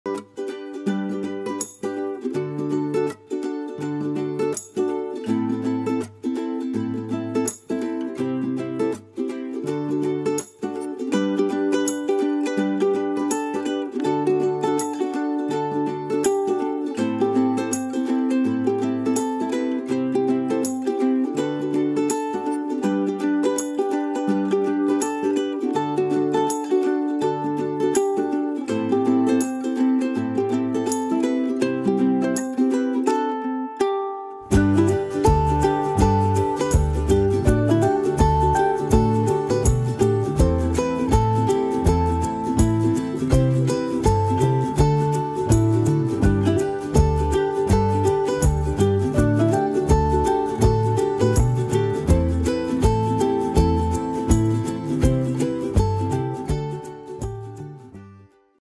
Teaser Ensaio Gestante
Um pouquinho do ensaio da Fran e do Hugo, esperando o Gabriel e o Miguel!